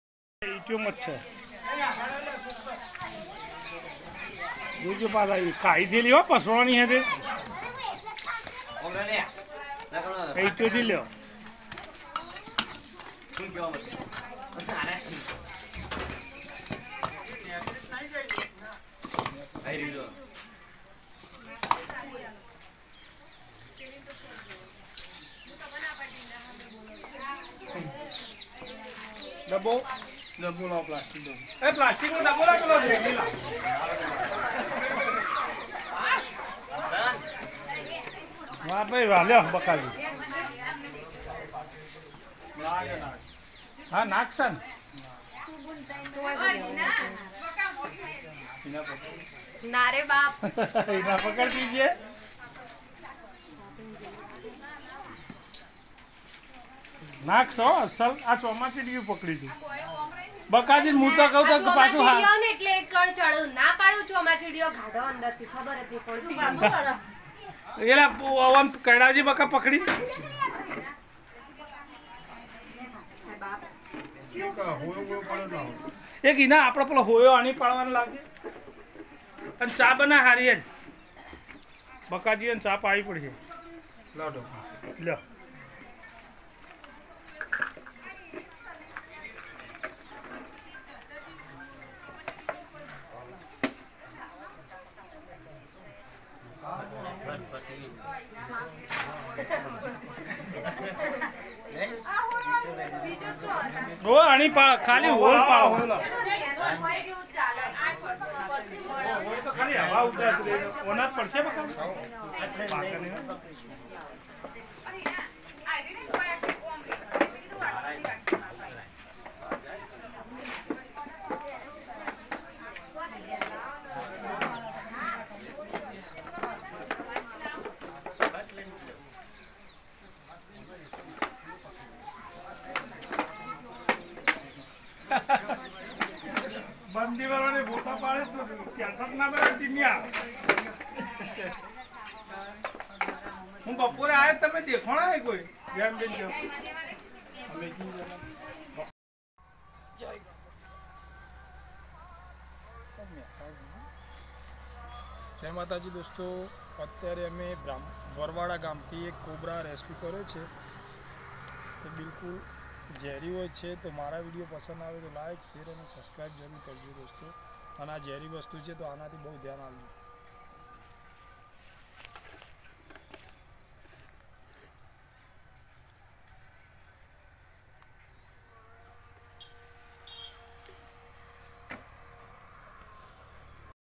(Indian spectacled cobra) (Naja Naja) દોસ્તો વરવાડા ગામથી પટેલ ના ઘરે થી ઝેરી સાપ પકડ્યો
દોસ્તો મારા વિડીયો પસંદ આવે તો લાઈક શેર અને સબસ્ક્રાઇબ જરૂર કરજો